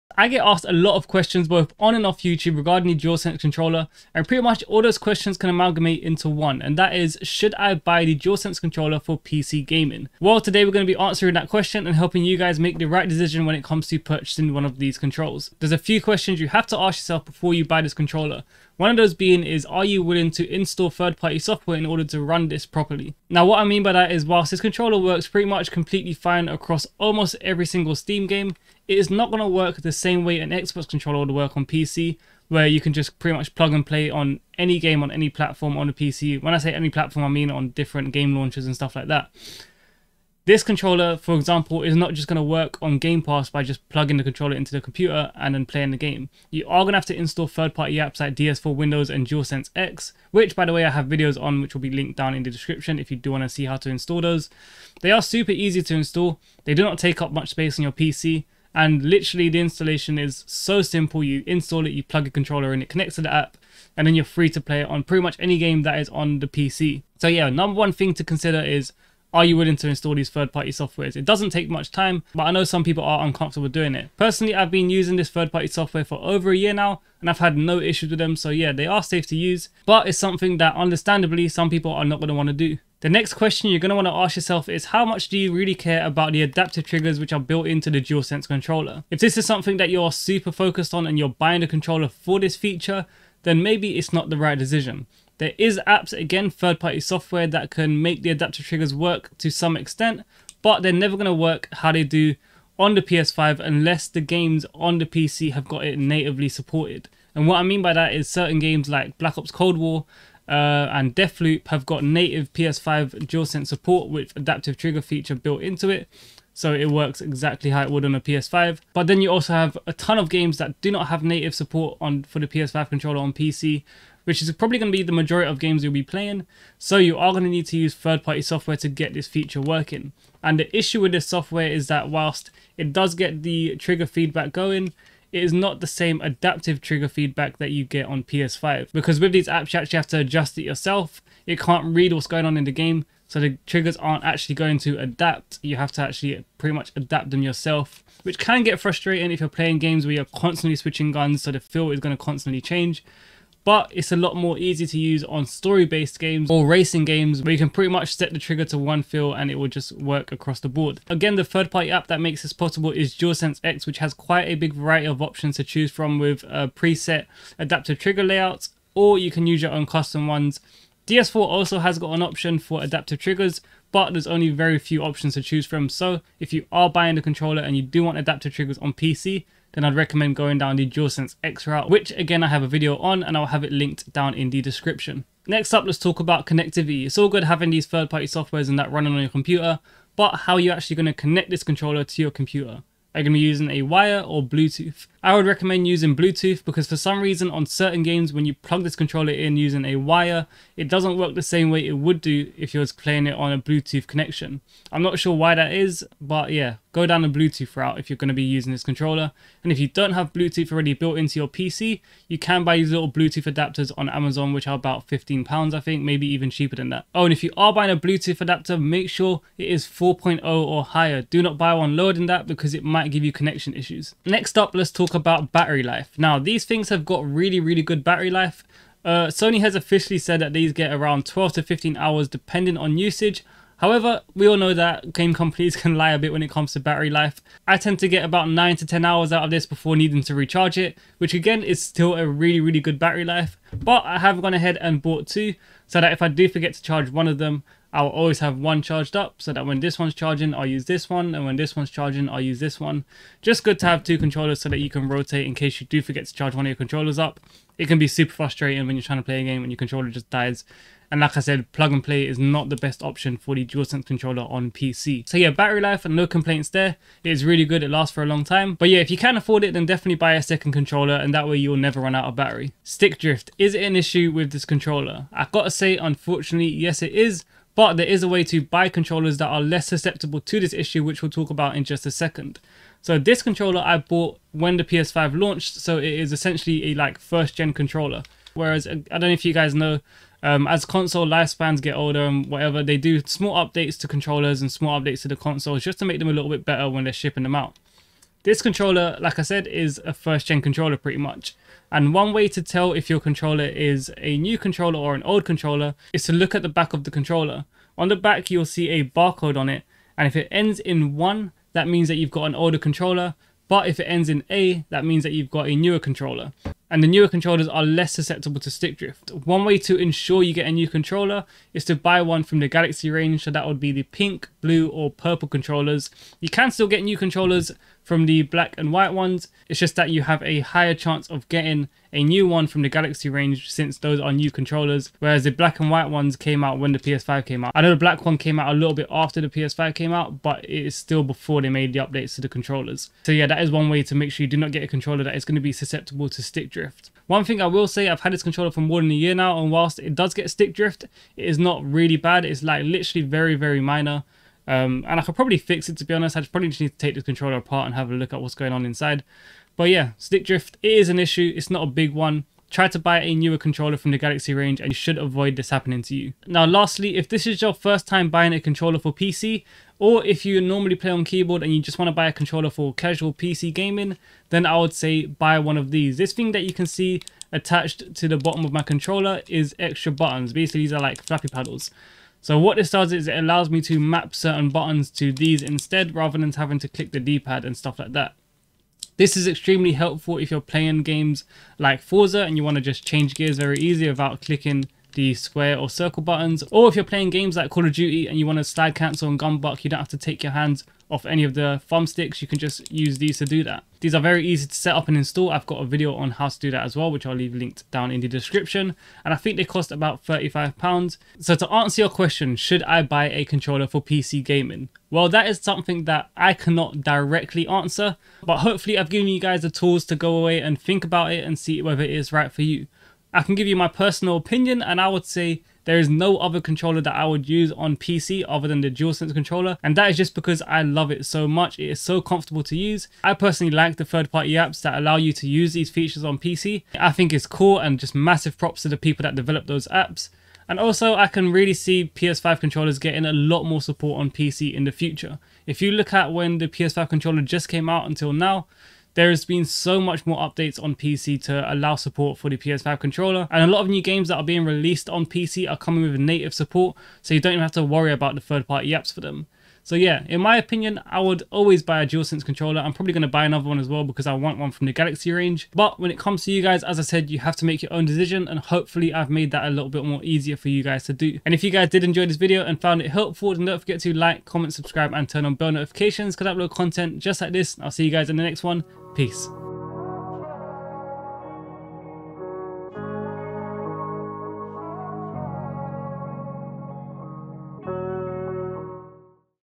Should You Buy a PS5 DualSense Controller For PC Gaming?
If you're thinking about buying a DualSense controller for PC then watch this video first.

USB Type C Cable
Bluetooth Adapter